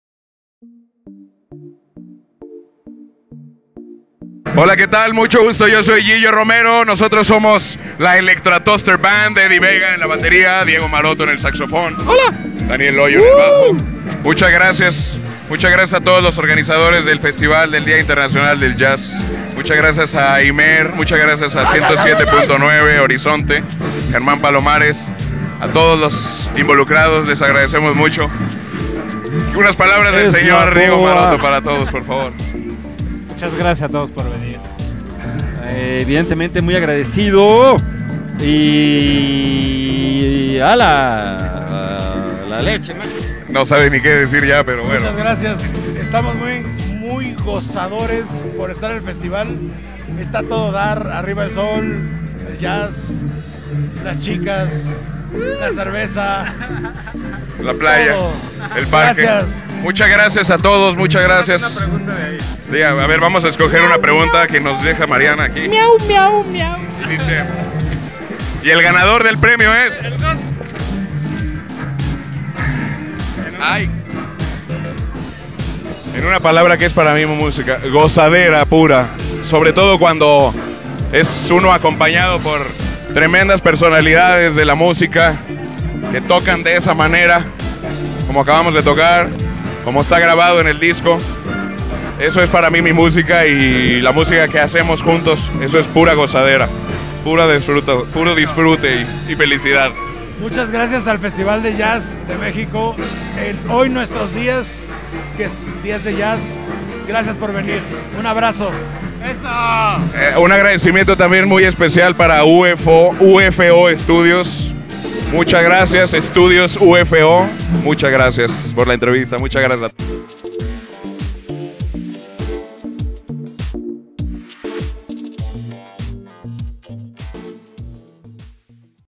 Festival internacional de Jazz - Gigio Romero
DIA INTERNACIONAL DEL JAZZ
International Jazz Day
Parque Hundido, Luis G.Urbina
28, 29 y 30 de abril de 2017.
Entrada Libre